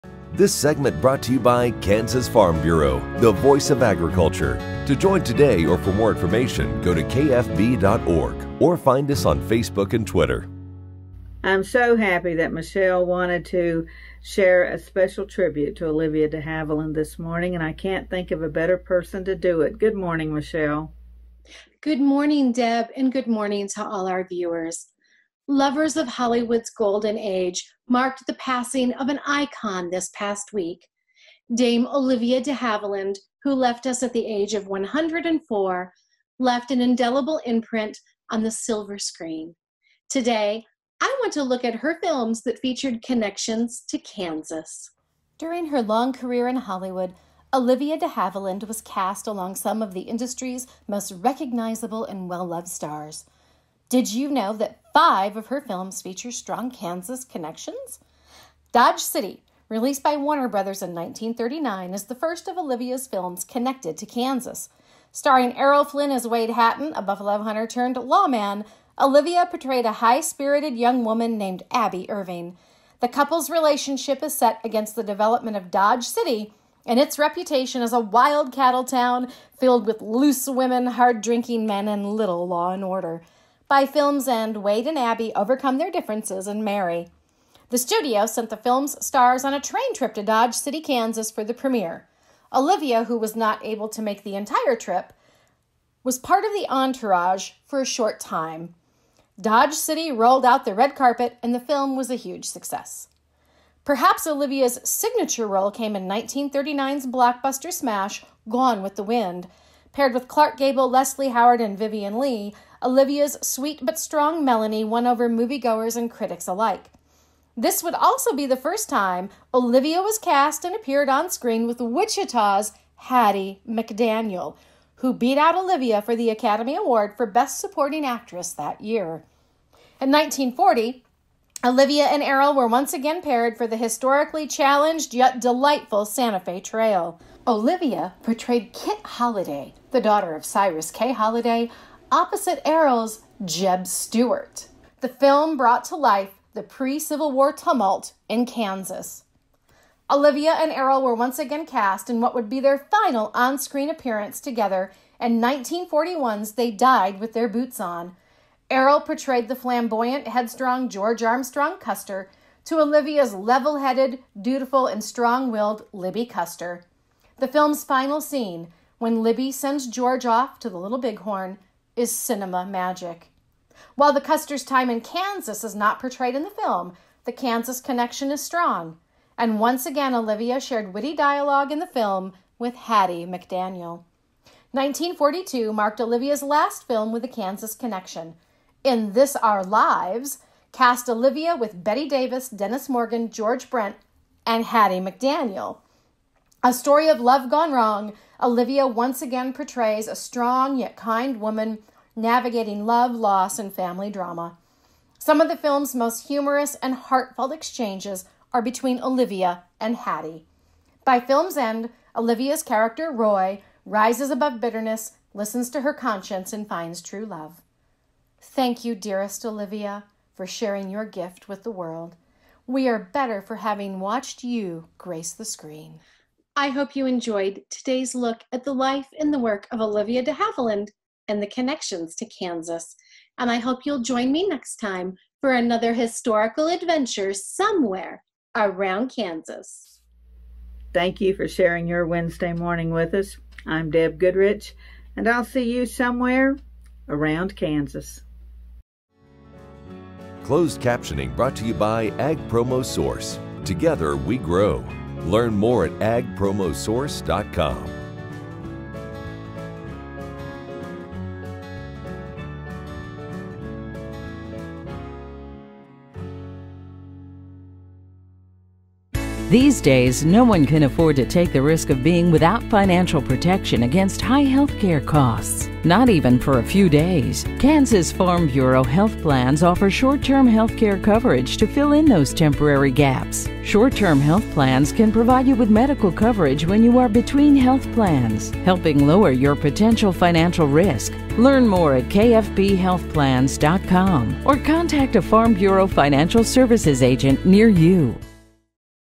tribute to Olivia de Havilland
I'm so happy that Michelle wanted to share a special tribute to Olivia de Havilland this morning and I can't think of a better person to do it. Good morning, Michelle.
Michelle: Good morning, Deb, and good morning to all our viewers. Lovers of Hollywood's golden age marked the passing of an icon this past week. Dame Olivia de Havilland, who left us at the age of 104, left an indelible imprint on the silver screen. Today, I want to look at her films that featured connections to Kansas.
During her long career in Hollywood, Olivia de Havilland was cast along some of the industry's most recognizable and well-loved stars. Did you know that five of her films feature strong Kansas connections?
Dodge City, released by Warner Brothers in 1939, is the first of Olivia's films connected to Kansas. Starring Errol Flynn as Wade Hatton, a buffalo hunter turned lawman, Olivia portrayed a high-spirited young woman named Abbie Irving. The couple's relationship is set against the development of Dodge City and its reputation as a wild cattle town filled with loose women, hard-drinking men, and little law and order. By film's end, Wade and Abby overcome their differences and marry.
The studio sent the film’s stars on a train trip to Dodge City, Kansas for the premiere. Olivia, who was not able to make the entire trip, was part of the entourage for a short time. Dodge City rolled out the red carpet and the film was a huge success.
Perhaps Olivia's signature role came in 1939's blockbuster smash, Gone With the Wind. Paired with Clark Gable, Leslie Howard, and Vivian Leigh, Olivia's sweet but strong Melanie won over moviegoers and critics alike. This would also be the first time Olivia was cast and appeared on screen with Wichita's Hattie McDaniel, who beat out Olivia for the Academy Award for Best Supporting Actress that year.
In 1940, Olivia and Errol were once again paired for the historically challenged yet delightful Santa Fe Trail. Olivia portrayed Kit Holliday, the daughter of Cyrus Kay Holliday, opposite Errol's Jeb Stuart. The film brought to life the pre-Civil War tumult in Kansas.
Olivia and Errol were once again cast in what would be their final onscreen appearance together in 1941's They Died With Their Boots On. Errol portrayed the flamboyant, headstrong George Armstrong Custer to Olivia's level-headed, dutiful, and strong-willed Libby Custer. The film's final scene, when Libby sends George off to the Little Bighorn, is cinema magic.
While the Custers' time in Kansas is not portrayed in the film, the Kansas connection is strong. Once again, Olivia shared witty dialogue in the film with Hattie McDaniel.
1942 marked Olivia's last film with a Kansas connection. In This Our Lives cast Olivia with Bette Davis, Dennis Morgan, George Brent, and Hattie McDaniel. A story of love gone wrong, Olivia once again portrays a strong yet kind woman navigating love, loss. and family drama. Some of the film’s most humorous and heartfelt exchanges are between Olivia and Hattie. By film's end, Olivia's character Roy rises above bitterness, listens to her conscience, and finds true love.
Thank you, dearest Olivia for sharing your gift with the world. We are better for having watched you grace the screen. I hope you enjoyed today's look at the life and the work of Olivia de Havilland and the connections to Kansas. I hope you'll join me next time for another historical adventure somewhere around Kansas.
Deb: Thank you for sharing your Wednesday morning with us. I'm Deb Goodrich and I'll see you somewhere around Kansas.